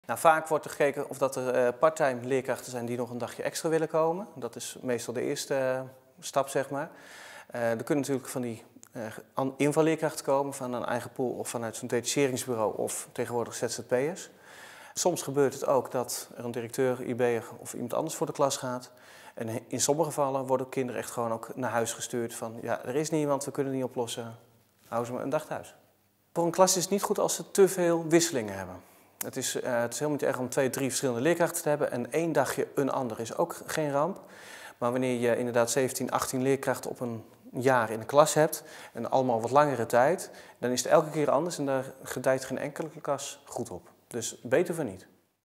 Eddy Erkelens over het lerarentekort (1)
Maandag beginnen de scholen weer. Veel kinderen in Noord-Holland schuiven dan weer aan in de klas van de vertrouwde juf of meester. Maar niet voor iedereen. Door het lerarentekort krijgen sommige kinderen wel achttien verschillende leerkrachten in één jaar.